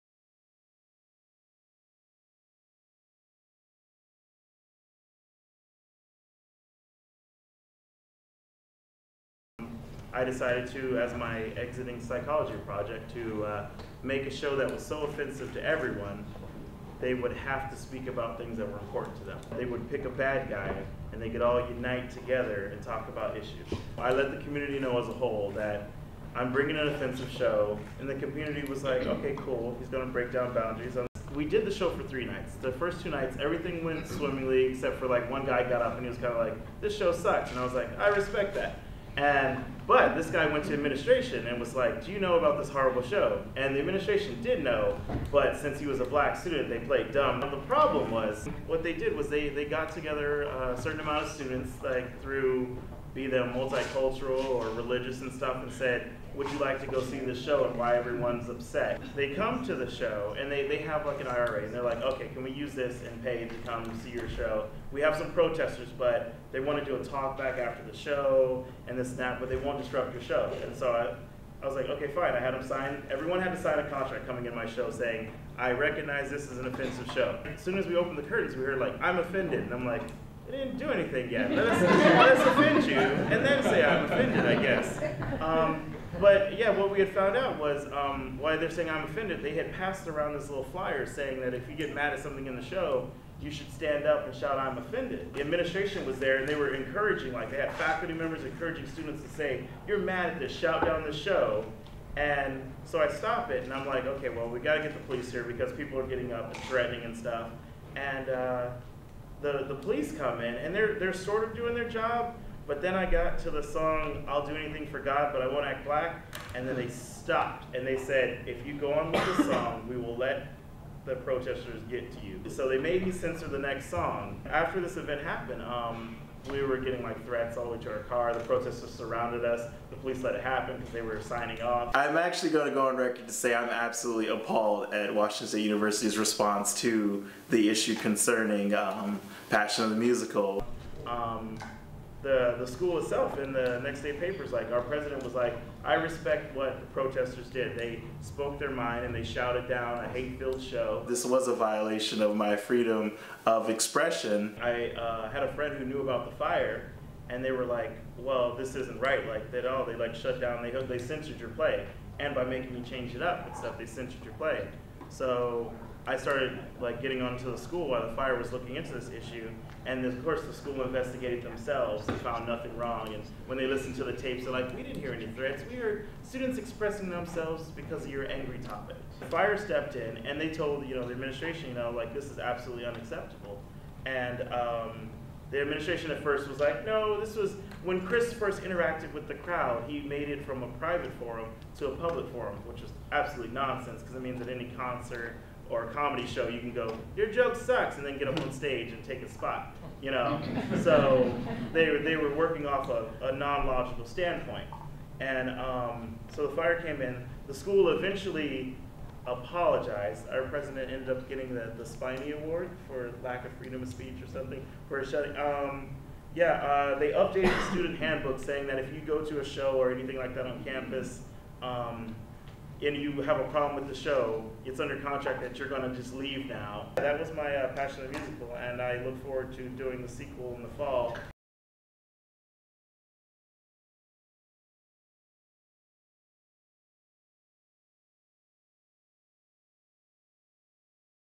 How Washington State Censored a Student Musical
In a new video from the 2010 Campus Freedom Network (CFN) Conference former Washington State University (WSU) student Chris Lee discusses how administrators at WSU subsidized a group of protesters to disrupt his satirical play, 'Passion of the Musical.'